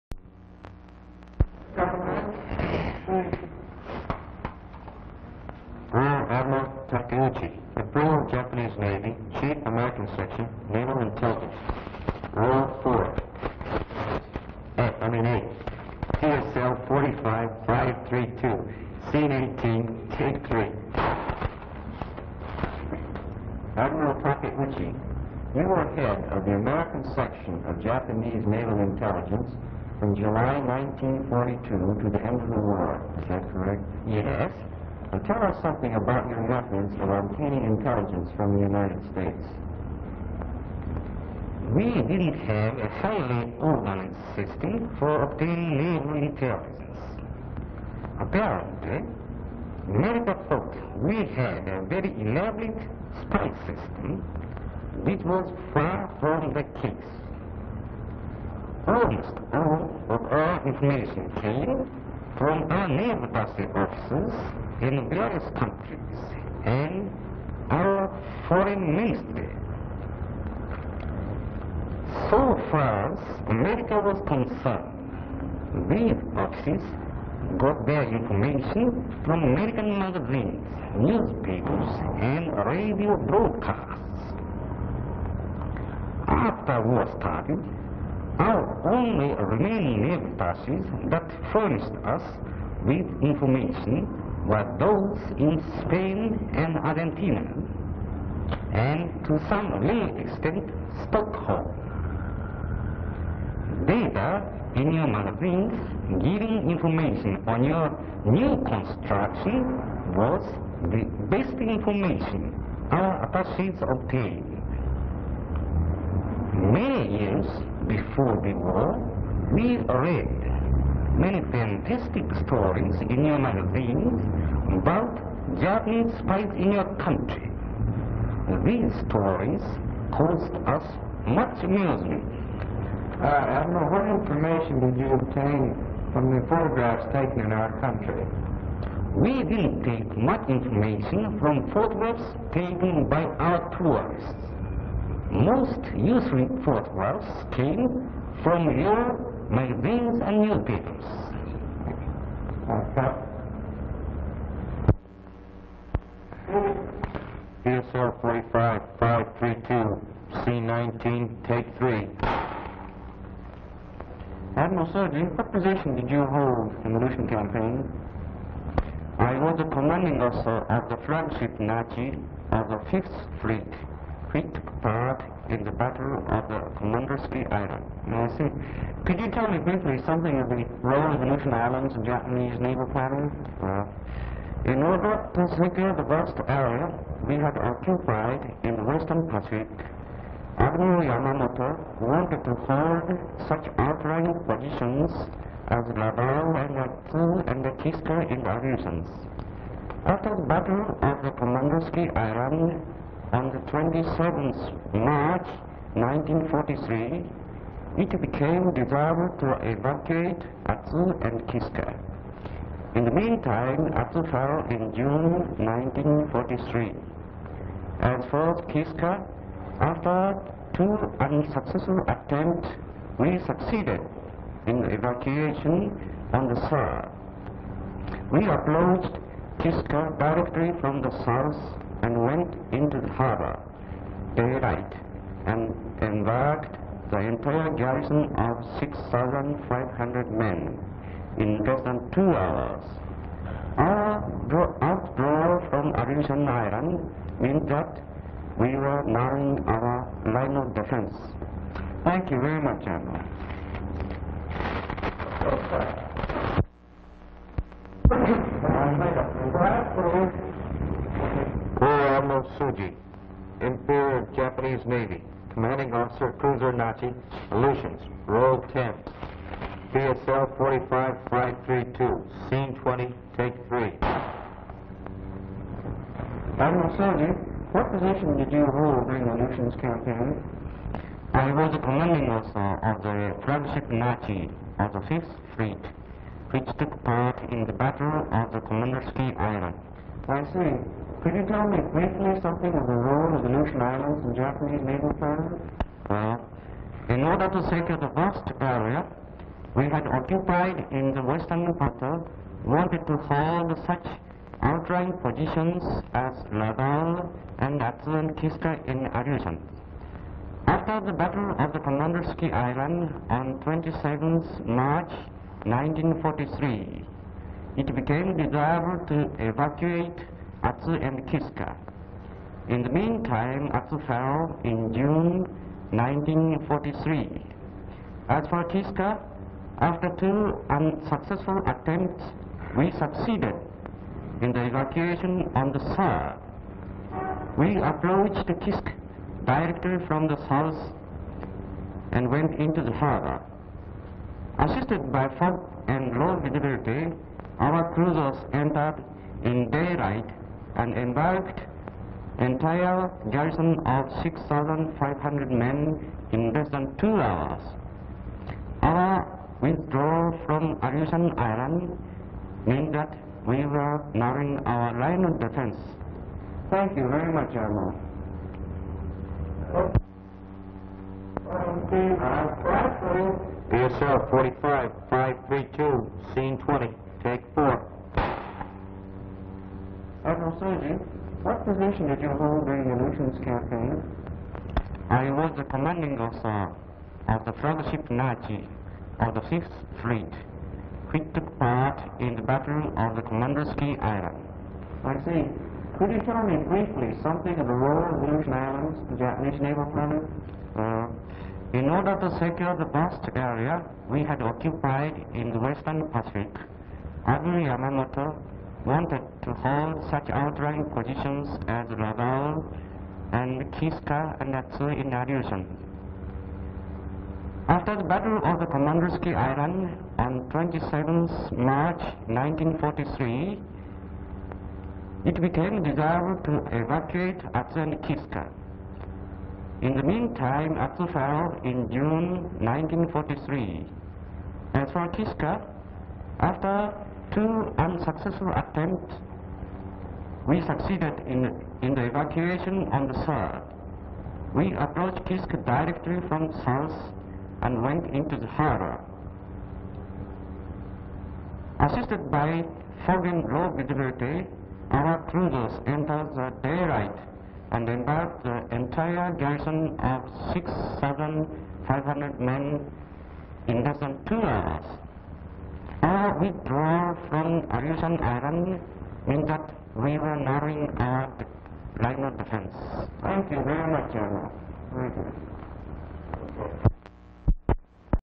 IMPERIAL JAPANESE NAVY 12-1945 ADMIRALS SOJI - TAKEUCHI - HD
Naval Photographic Center film #10630. description LS Rear Admiral A. Soji, Commanding Officer of Cruiser Nachi (Aleutians and Rear Admiral Kaoru Takeuchi; Imperial Japanese Navy, Chief Adm. Sec. Naval Intelligence. CU Admiral Soji.
Education & memory purposes